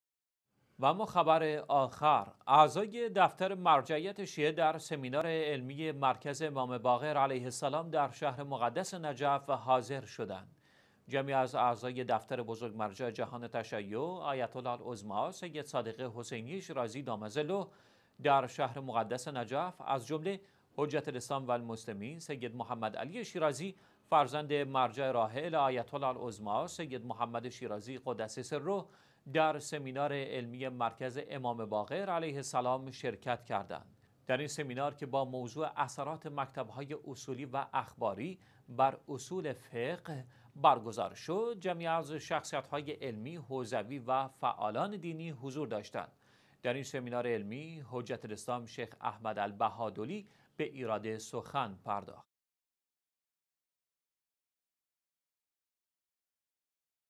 حضور اعضای دفتر مرجعیت در سمینار «اثرات مکتب اصولی و اخباری بر اصول فقه»
اعضای دفتر مرجعیت شیعه، در سمینار علمی مرکز امام باقر علیه السلام در شهر مقدس نجف حاضر شدند.

جمعی از اعضای دفتر بزرگ مرجع جهان تشیع آیت الله العظمی سید صادق حسینی شیرازی (دام ظله) در شهر مقدس نجف، از جمله حجت الاسلام و المسلمین «سید محمد علی شیرازی» فرزند مرجع راحل آیت الله العظمی سید محمد شیرازی (قدس سره) در سمینار علمی مرکز امام باقر علیه السلام شرکت کردند.
در این سمینار که با موضوع «اثرات مکتب‌های اصولی و اخباری بر اصول فقه»  برگزار شد، جمعی از شخصیت‌های علمی حوزوی و فعالان دینی حضور داشتند.
در این سمینار علمی حجت الاسلام «شیخ احمد البهادلی» به ایراد سخن پرداخت.